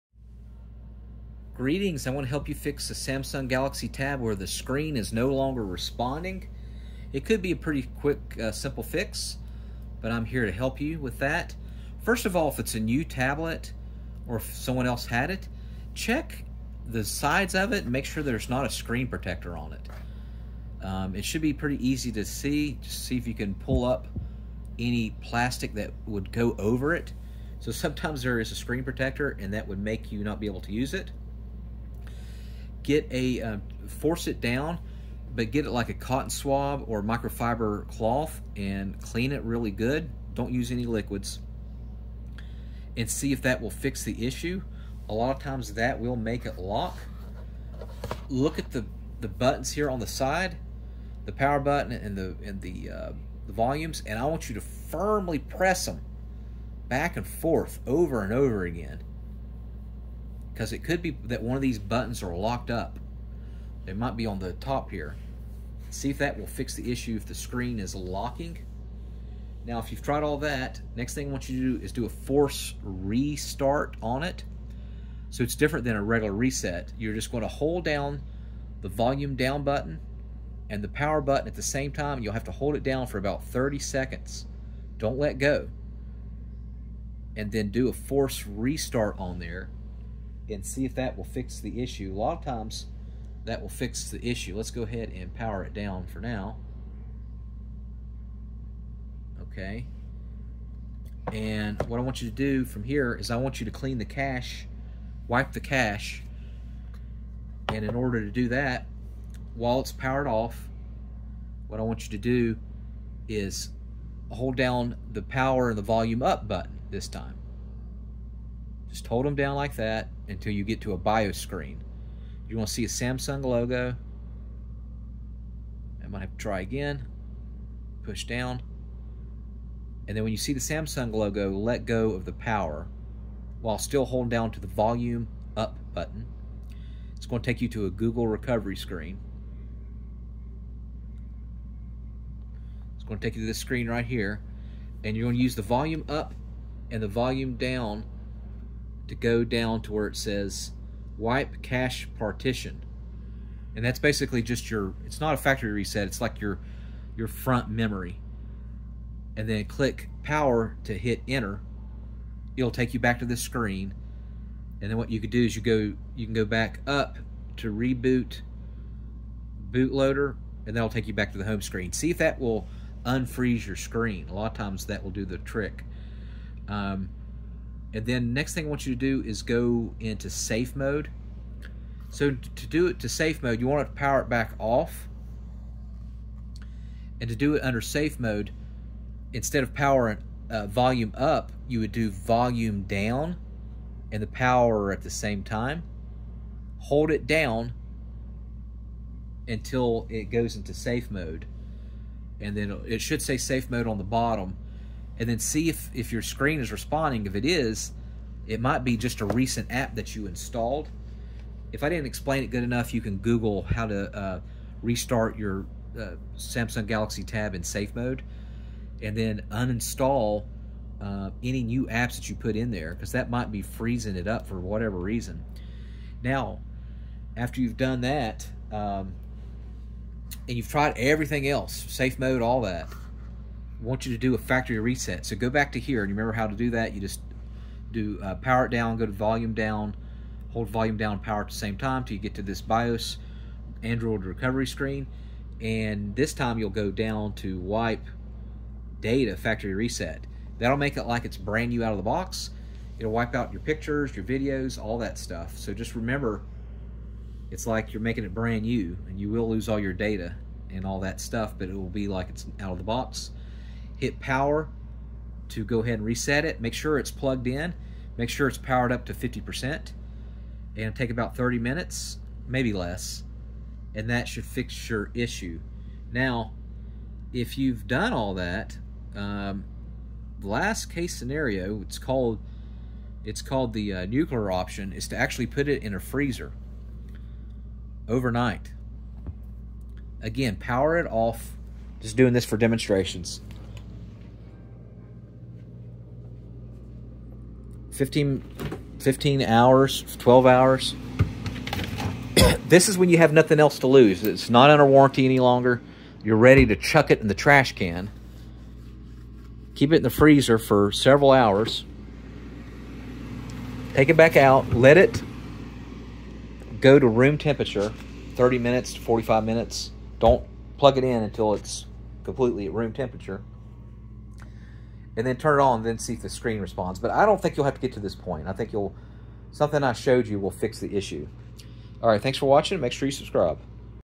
Fix Samsung Galaxy Tab SCREEN No Longer Responding to Touch (Tablet Wont Move S8 S8+ S9 FE S10 S10+)
If your Samsung Galaxy Tab is no longer responding to touch, try these troubleshooting steps:
1. Restart the Device:
Press and hold the Power button for about 10 seconds until the device restarts. This can help clear minor software issues.
2. Force Restart:
If the screen is completely unresponsive, press and hold the Power button and Volume Down button together for about 10–20 seconds. This will force the tablet to restart.
3. Clean the Screen:
Sometimes dirt or moisture can cause the touchscreen to become unresponsive. Wipe the screen with a clean, dry cloth.
4. Remove Any External Accessories:
If you have any cases, screen protectors, or other accessories on the tablet, try removing them. Some accessories may interfere with the touchscreen's functionality.
5. Boot into Safe Mode:
Turn off the tablet.
Turn it on, and as soon as the Samsung logo appears, press and hold the Volume Down button.
Keep holding it until the device finishes booting into Safe Mode. If the touchscreen works in Safe Mode, third-party apps are likely causing the issue. Uninstall any recently added apps that might be interfering with the touchscreen.
6. Wipe Cache Partition:
Press and hold the Volume Up + Power buttons together until the Samsung logo appears.
Once the recovery menu appears, use the Volume buttons to navigate to Wipe Cache Partition, and select it with the Power button.
Once completed, select Reboot system now.
7. Factory Reset (as a last resort):
If nothing else works, a factory reset may be necessary. This will erase all data on the tablet, so back up important information if possible.
Navigate to Wipe data/factory reset and select it with the Power button.
Confirm the reset by selecting Yes.
Once complete, select Reboot system now.
8. Check for Hardware Issues:
If the issue persists after trying these methods, there may be a hardware problem with the touchscreen, and you may need to contact Samsung support or visit a service center for further assistance.